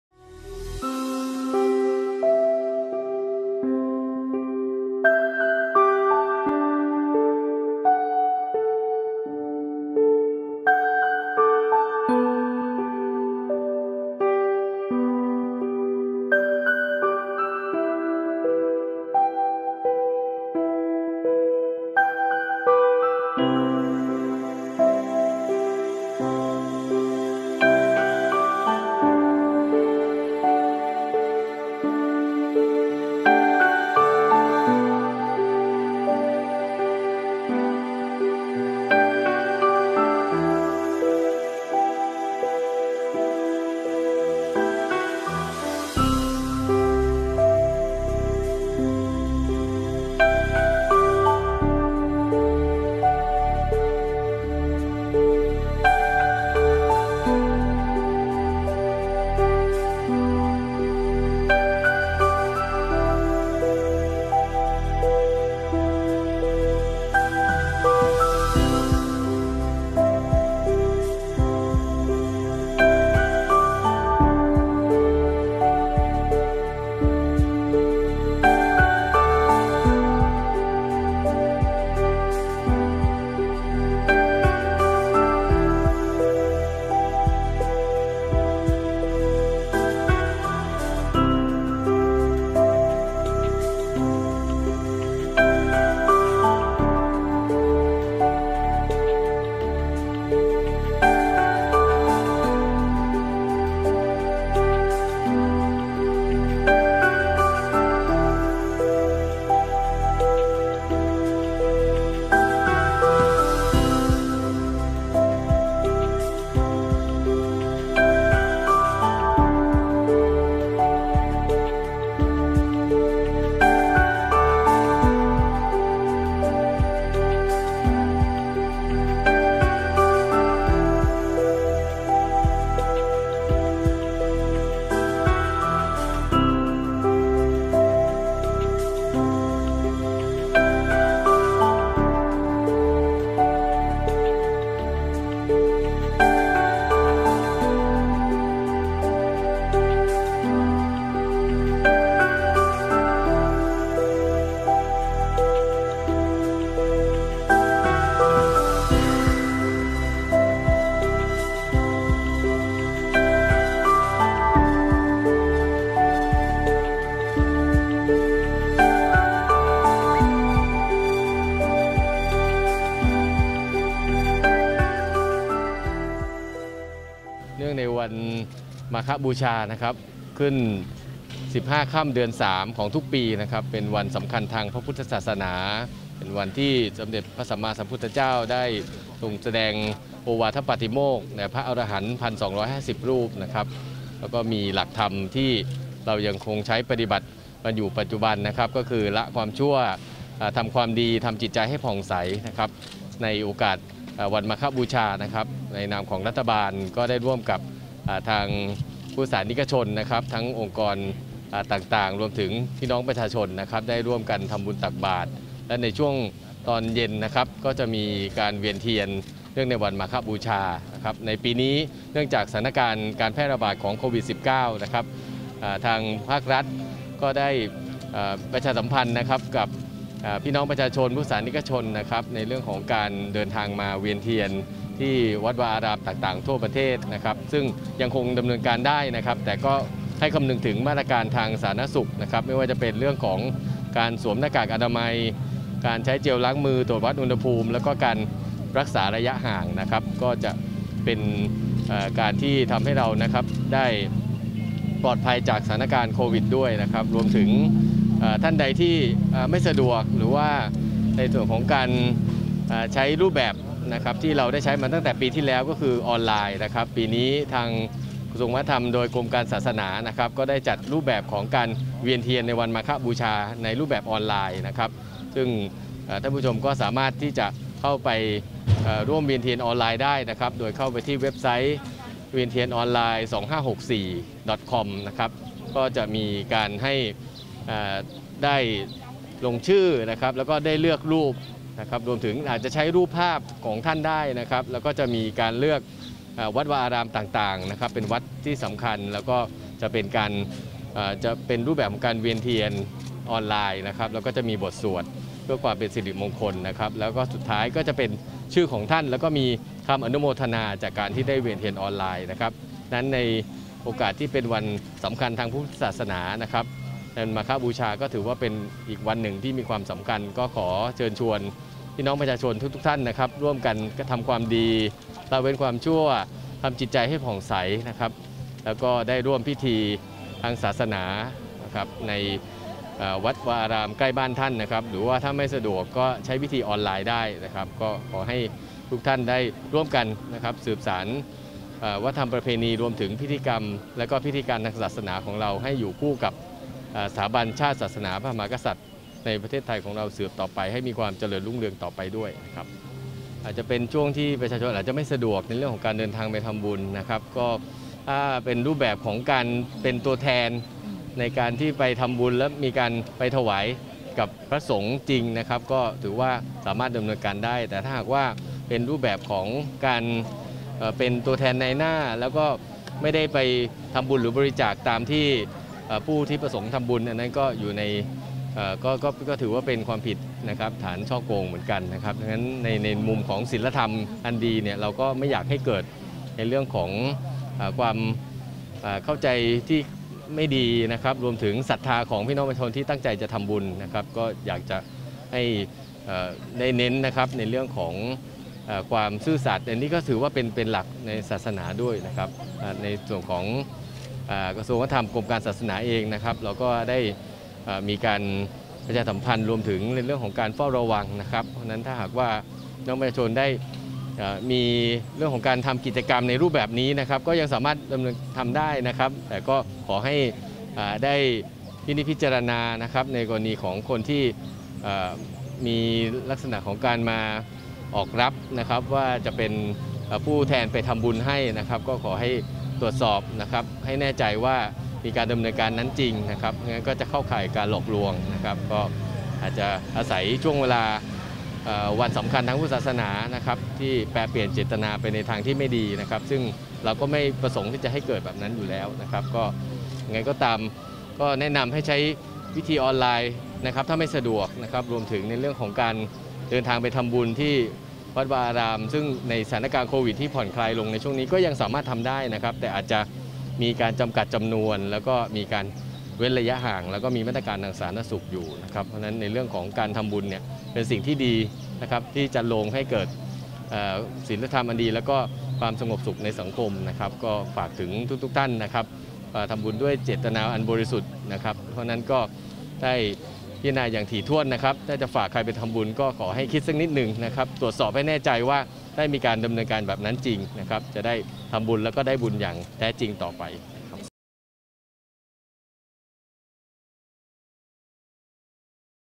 พิธีเจริญพระพุทธมนต์ ตักบาตร และเวียนเทียน เนื่องในเทศกาลวันมาฆบูชา พุทธศักราช ๒๕๖๔
วันศุกร์ที่  ๒๖ กุมภาพันธ์ ๒๕๖๔  เวลา ๐๗.๐๐ น. นายอิทธิพล คุณปลื้ม รัฐมนตรีว่าการกระทรวงวัฒนธรรม เป็นประธานพิธีเจริญพระพุทธมนต์ ตักบาตร และเวียนเทียน เนื่องในเทศกาลวันมาฆบูชา พุทธศักราช ๒๕๖๔ โดยมี นายปรเมศวร์ งามพิเชษฐ์ ผู้ช่วยรัฐมนตรีประจำกระทรวงวัฒนธรรม นางยุพา ทวีวัฒนะกิจบวร ปลัดกระทรวงวัฒนธรรม นายพิกิฏ ศรีชนะ เลขานุการรัฐมนตรีว่าการกระทรวงวัฒนธรรม ผู้บริหาร ข้าราชการ และพุทธศาสนิกชน เข้าร่วมพิธี ณ วัดสระเกศราชวรมหาวิหาร (ภูเขาทอง) กรุงเทพฯ